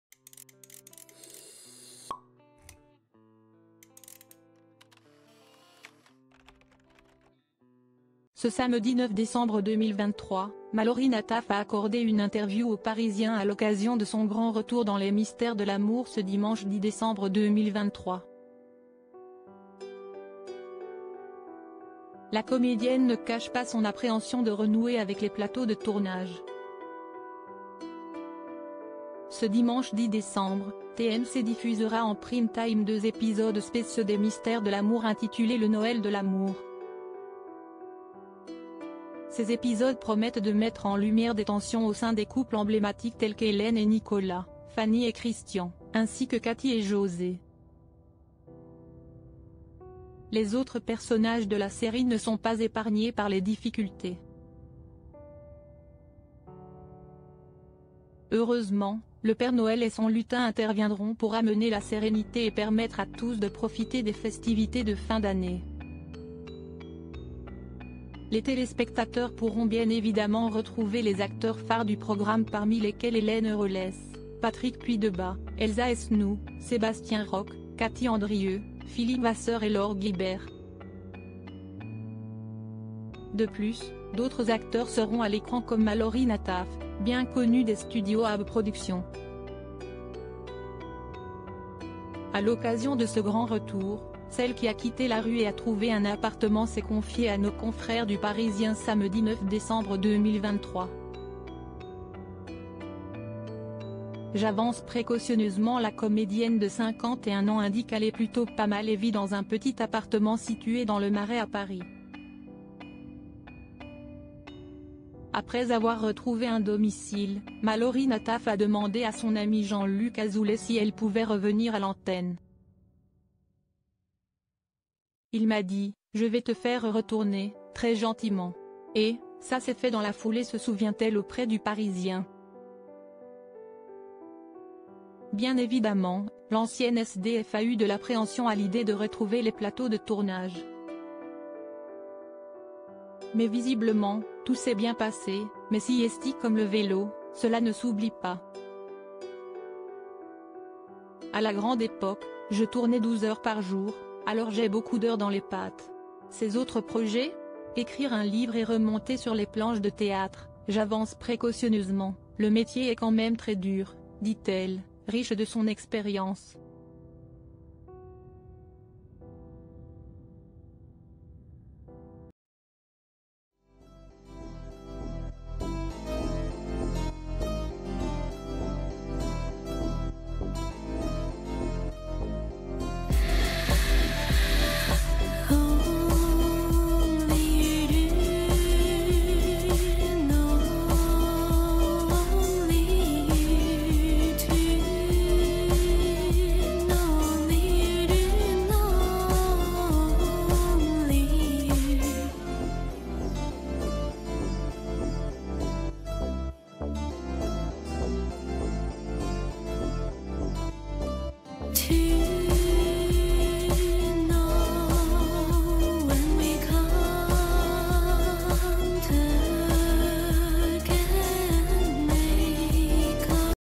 Les Mystères de l’amour Mallaury Nataf révèle avoir “eu de l’appréhension” à l’idée de faire son r
Les Mystères de l’amour : Mallaury Nataf révèle avoir “eu de l’appréhension” à l’idée de faire son retour à la télévision